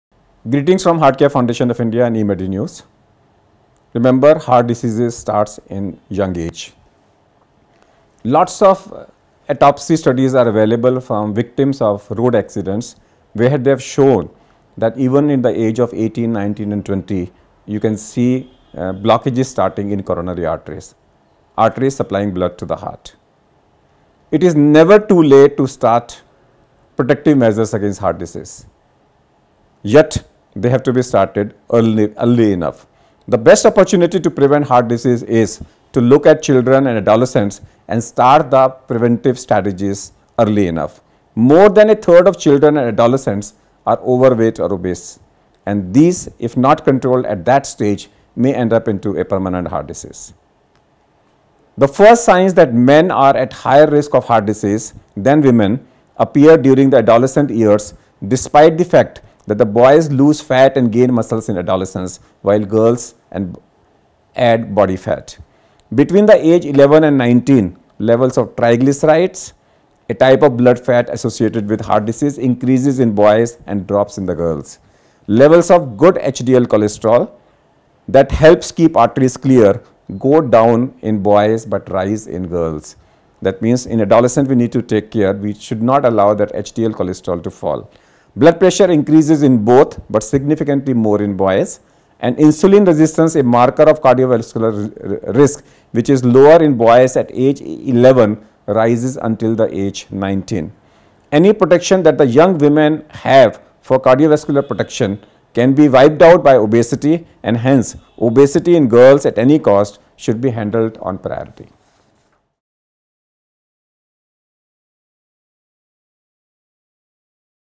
Heart disease starts in youth
Padma Shri and Dr B C Roy National Awardee Dr KK Aggarwal on Heart disease starts in youth